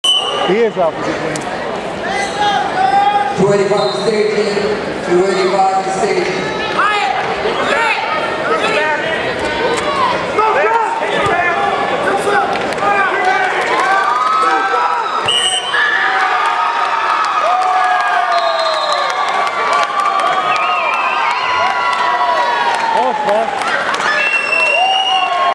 2015 TOC 220 Pounds Final
Josh Ozuna of Liberty (NV) vs Andrew Ramos of El Dorado